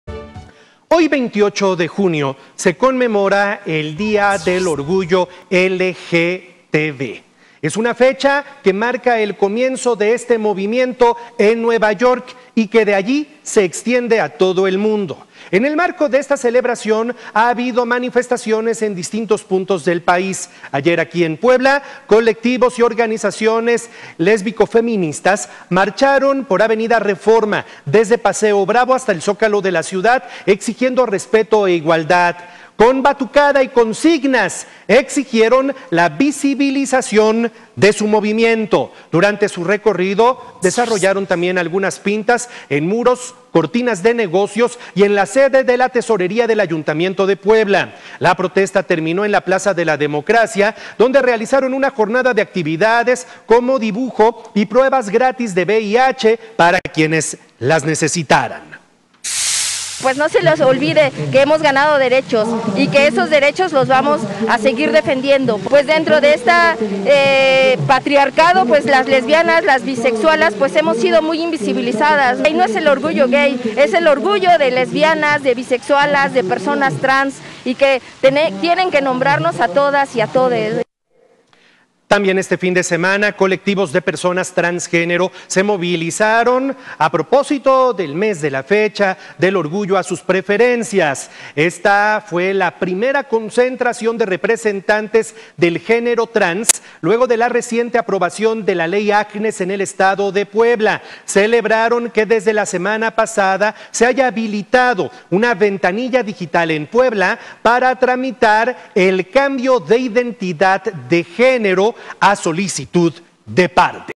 Celebran día del orgullo || Noticias con Juan Carlos Valerio
Este día se celebra el día del Orgullo LGBT y ayer en Puebla colectivos marcharon exigiendo sus derechos.